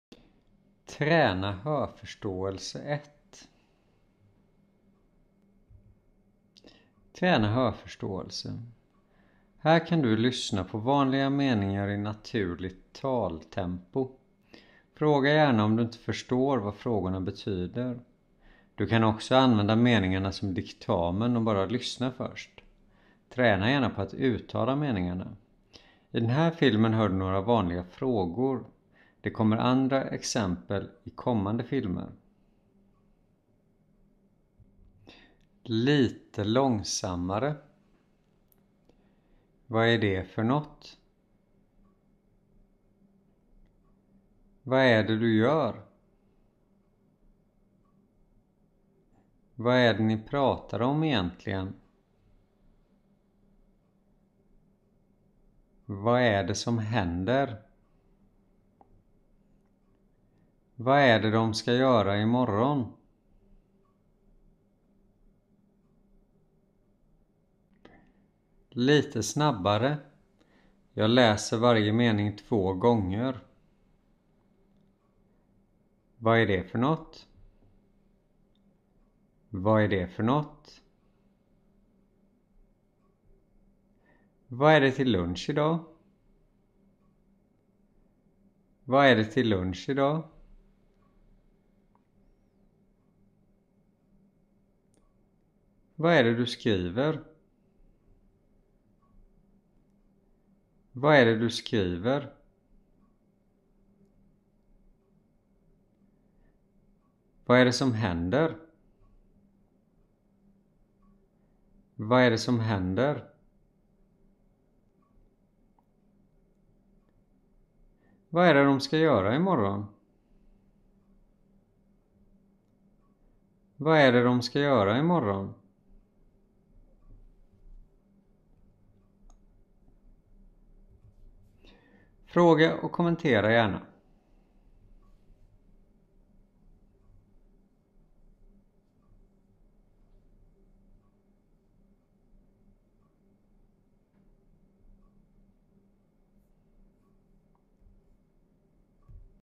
Hörförståelse 1 - Blandade vanliga meningar för att träna hörförståelse på svenska
Här kan du lyssna på vanliga meningar på svenska som jag säger ganska snabbt. Du kan bara läsa och lyssna, träna ditt uttal eller använda som diktamensövningar. Fråga om något är svårt att förstå.